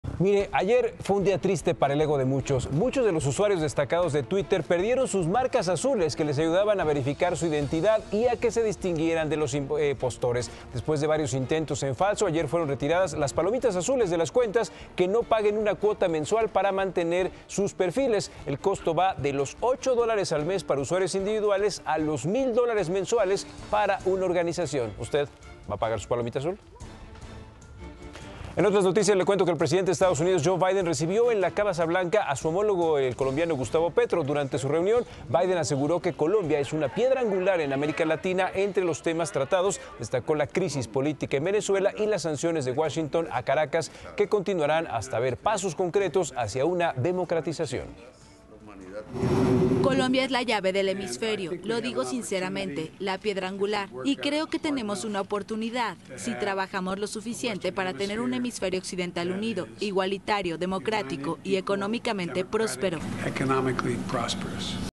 Twitter elimina la insignia azul de verificación | Noticias con Francisco Zea
¡Adiós a la palomita azul! Usuarios de Twitter perdieron sus marca azules que les ayudaban a verificar su identidad.